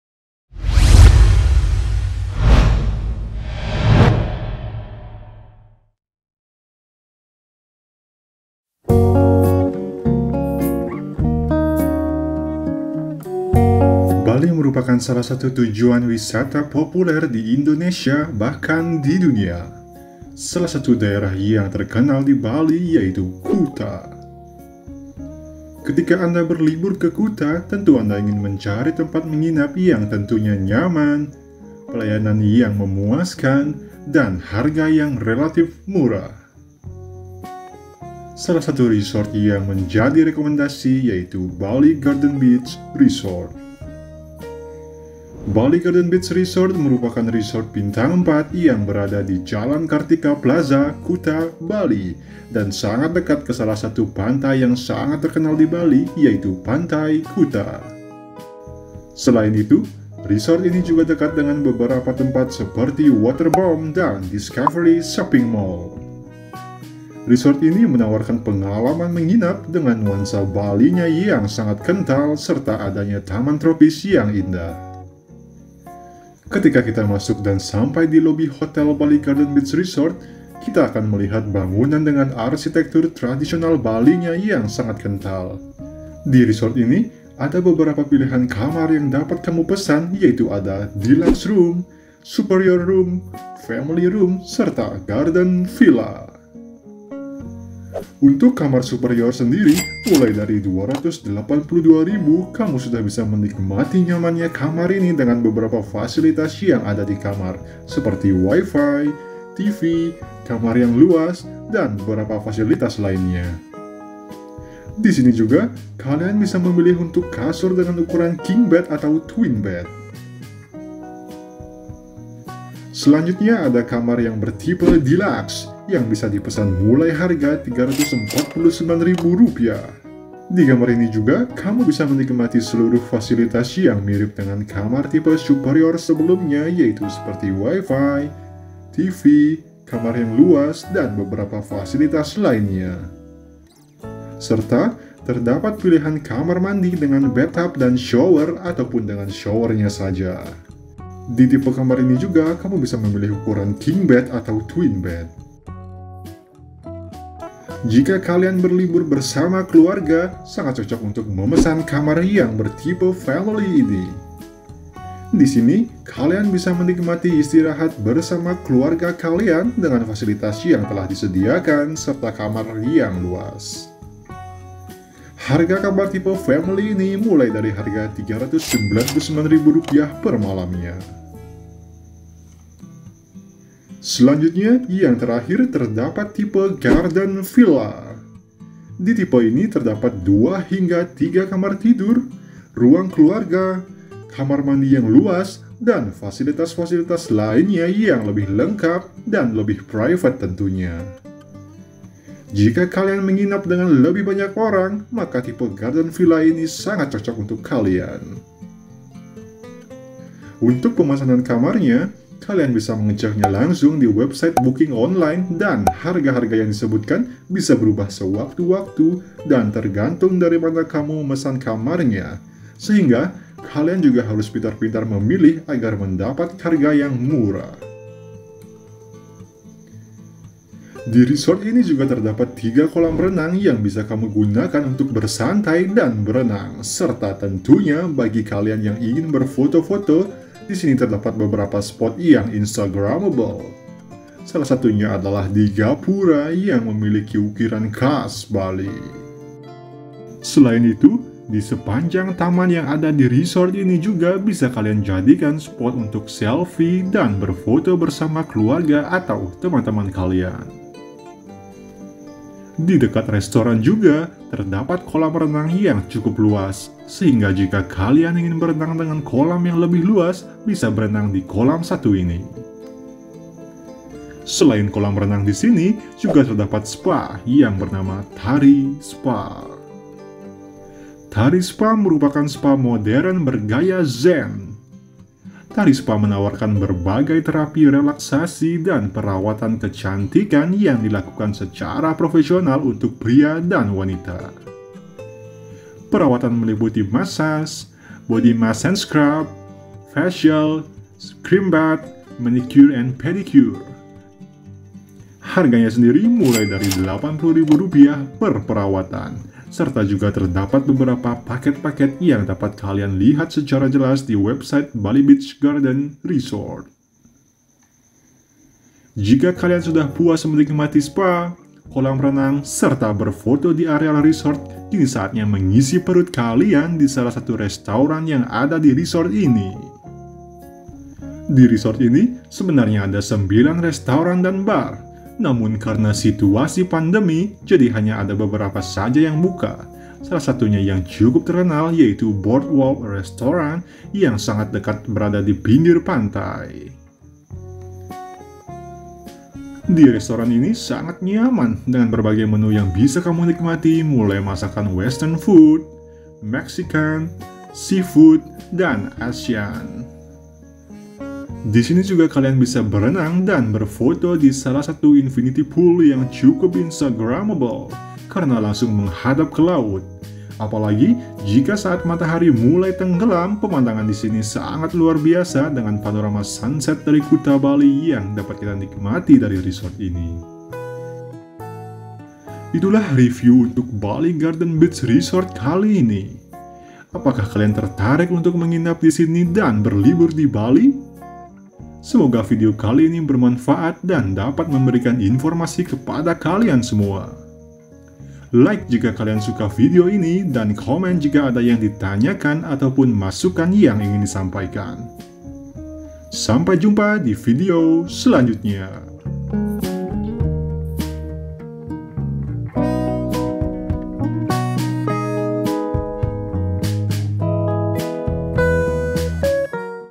SALAH SATU REKOMENDASI RESORT TERBAIK DI KUTA BALI ? - BALI GARDEN BEACH RESORT
Bali merupakan salah satu tujuan wisata populer di Indonesia bahkan di Dunia, salah satu daerah yang terkenal di Bali yaitu Kuta. Ketika anda berlibur ke Kuta tentu anda ingin mencari tempat menginap yang tentunya nyaman, pelayanan yang memuaskan dan harga yang relative murah. Salah satu resort yang menjadi rekomendasi yaitu Bali Garden Beach Resort.
Bali Garden Beach Resort merupakan resort bintang 4 yang berada di jalan Kartika plaza, kuta, bali dan sangat dekat ke salah satu pantai yang sangat terkenal di Bali yaitu pantai Kuta, selain itu resort ini juga dekat dengan beberapa tempat seperti Waterboom dan Discovery Shopping Mall. Resort ini menawarkan pengalaman menginap dengan nuansa bali nya yang kental serta adanya taman tropis yang indah.
Akomodasi yang ditawarkan oleh resort ini yaitu Deluxe room, Superior Room, Family room dan Garden villa.
Serta terdapat beberapa fasilitas yang ada di hotel ini seperti Restaurant, Spa, Meeting Room, Gym, Swimming Pools dan Concierge Service.
Untuk harga resort bisa berubah sewaktu-waktu sehingga kalian yang ingin menginap di resort ini senantiasa harus melihat harganya langsung di beberapa website booking online ataupun di website resortnya langsung.
Tertarik untuk menginap di resort ini dan berlibur di Bali?

Bali Garden Beach Resort
Address: Jl. Kartika Plaza, Kuta, Kabupaten Badung, Bali 80361

Time Stamp:
00:42 Bali Garden Beach Resort
01:20 Lobby
01:40 Superior Room
02:06 Deluxe Room
02:43 Family Room
03:08 Garden Villa
04:06 Photo Spots
04:25 Swimming Pool
04:38 Tari Spa
05:47 BoardWalk Restaurant
06:15 Kuta's Sunset
06:23 Closing
V&D Media - An Interesting Media
Follow our Instagram account @vndmedia to get more updates everyday!